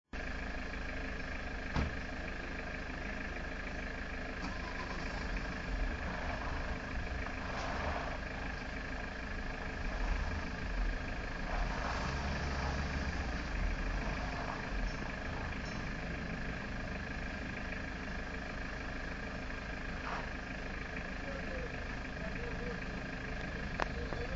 almost a funny (bad) accident
it took them one hour and half to get the car out.
they tried several methods, finaly a 4x4 came by
and yoinked him out.